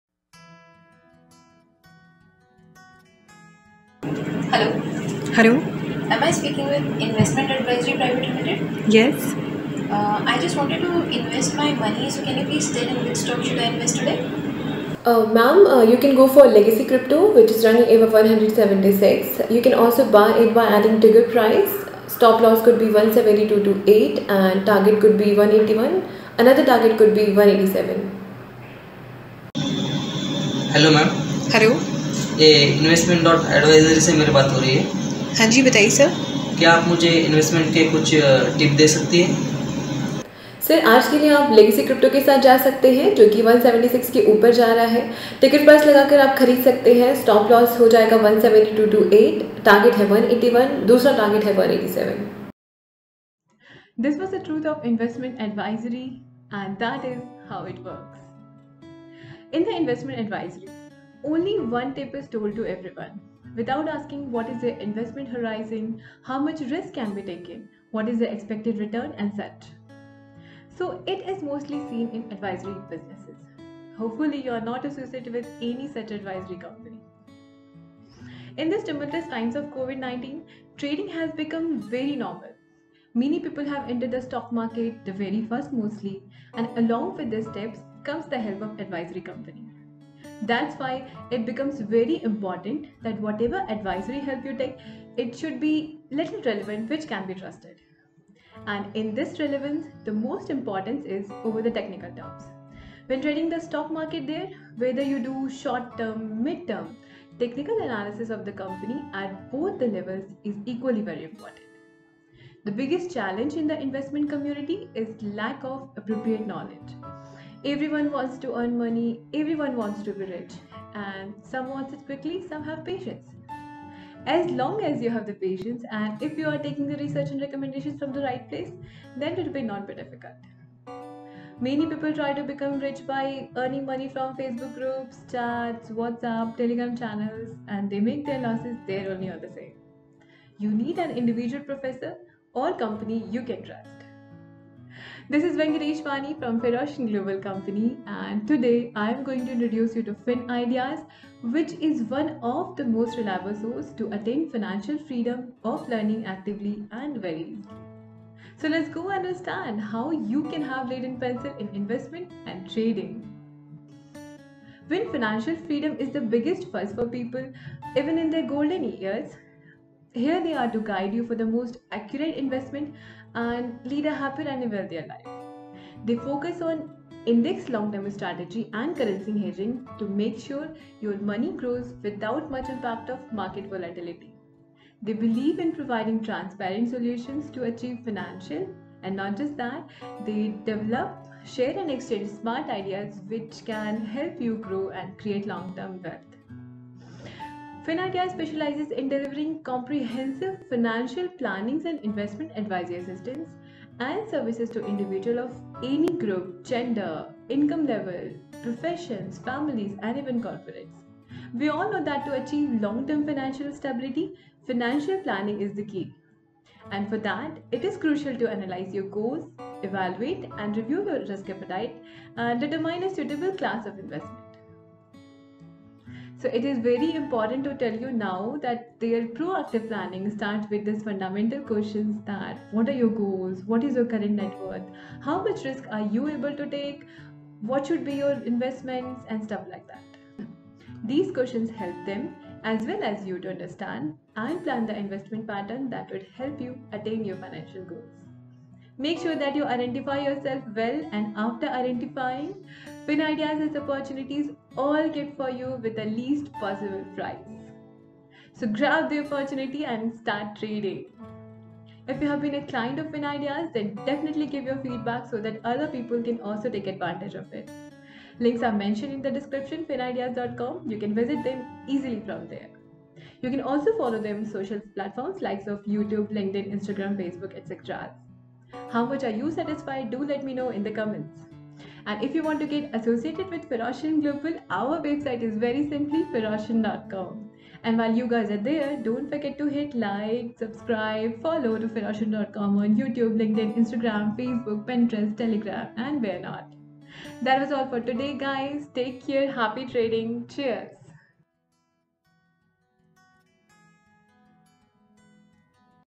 How Investment Frauds were Controlled in India? How Companies Like Finideas Saved People from Traps?
In this video, our Pharoscions are demonstrating the ways through which many stock market investment companies make people fool and put their money in market without thorough research and understanding of person's needs.

The second part of the video talks about Finideas (finideas.com), a company based in India which focuses on strategic investment planning and helps customers achieve their end goals with investing in index funds.